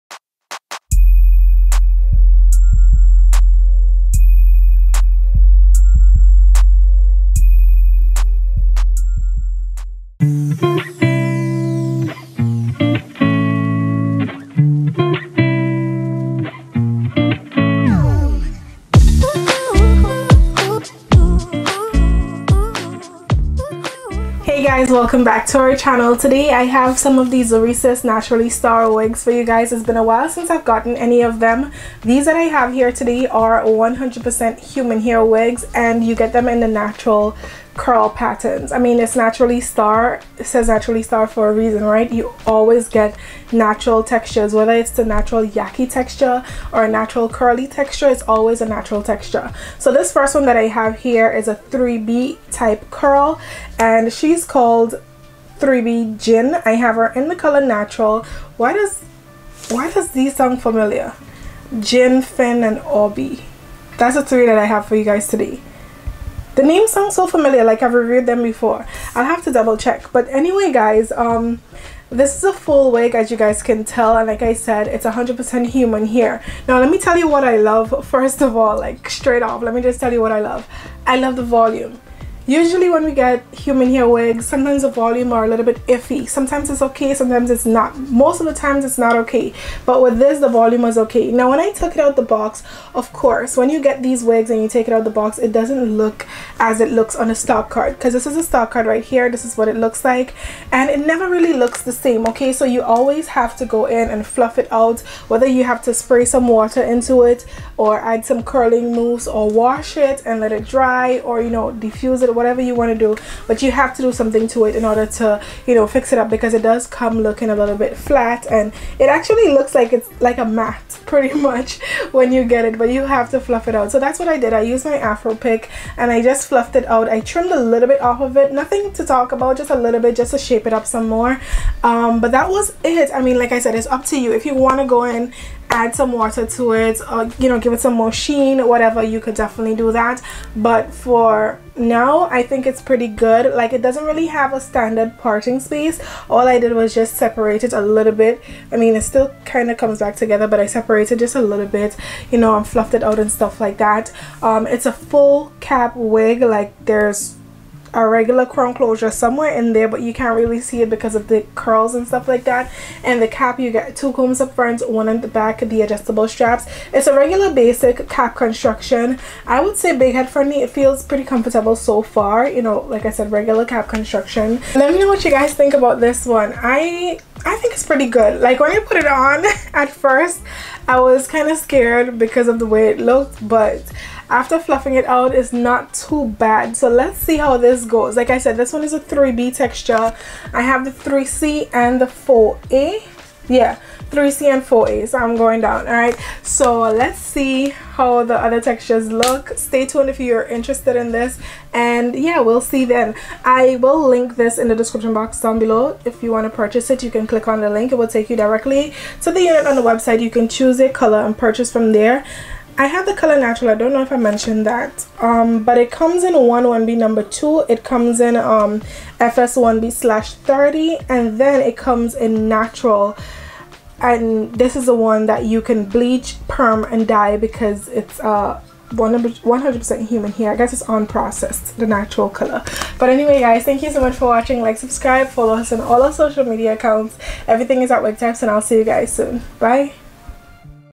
Zury Sis Naturali Star 100% Human Hair Wig - HR NAT 3B GIN --/WIGTYPES.COM
COLOR: NATURAL

*Full wig
*100% human hair
*3B curl texture
*Can be washed
*Big head friendly

AVAILABLE COLORS:
1, 1B, 2, FS1B/30, NATURAL

**TIME STAMPS**
0:00 Welcome / 360
0:24 Intro
1:12 Wig Features & Styling
2:54 Final Thoughts